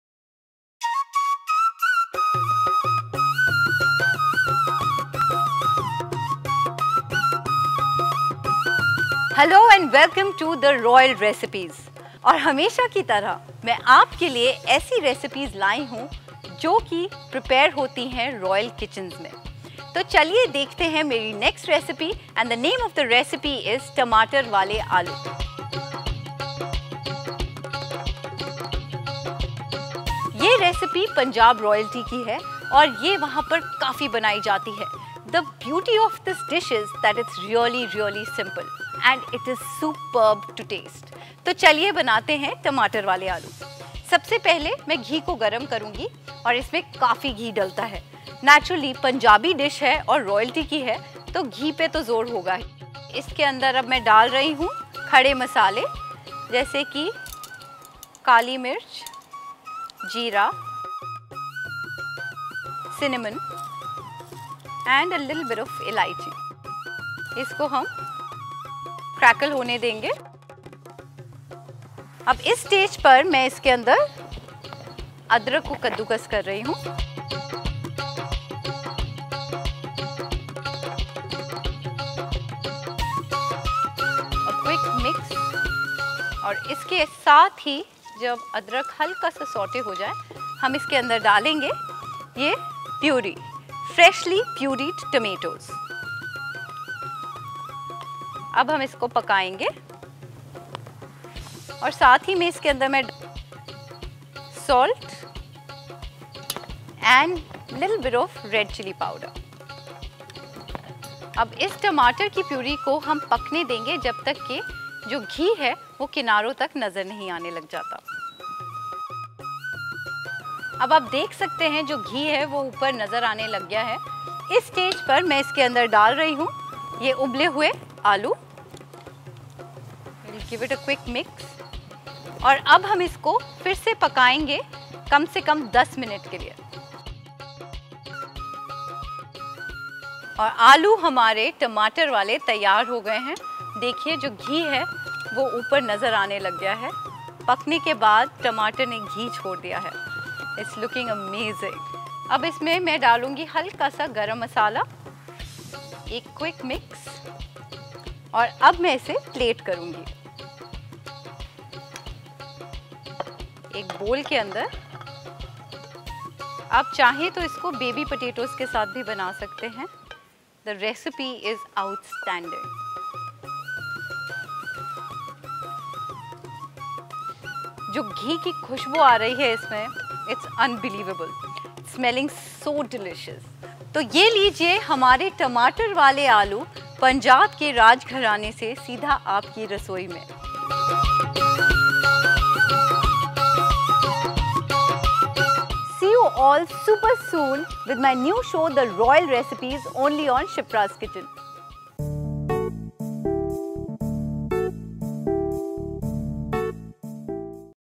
Tamatar Wale Aloo🍅🥔| MasterChef Shipra Khanna| Special Navratri series
In all households mostly eaten tamatar wale  aloo is a delicious chatpata recipe which is all time favorite and a hearty meal for the family.All basic spices and vegetables required for this easy to make recipe. The recipe is outstanding. Enjoy.

Ingredients:
2 tsp ghee
7 to 8 black pepper
2to 3 cinnamon sticks
1/2 tsp jeera
4 boiled potato
1 tsp salt
1 cup  tomato puree
1 tsp grated ginger
1/2 tsp garam masala

Method:
heat the pan and pour ghee, put all the dry masala and heat it till they crackle. Add puree and heat it for some time. till the time ghee starts leaving the pan base.. Then mix potato and heat it for some time. Now ready to serve by sprinkling garam masala and then plate it..enjoy.

TAG ME when cooking with my recipes :)